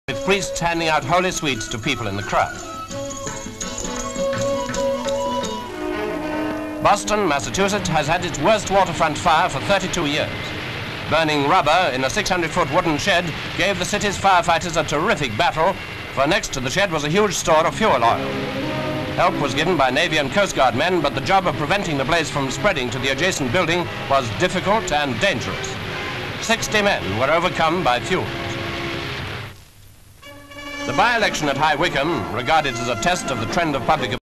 NEWS BRIEFS - BOSTON FIRE
(10 Nov 1952) Boston, Massachusetts, has had its worst waterfront fire for thirty-two years.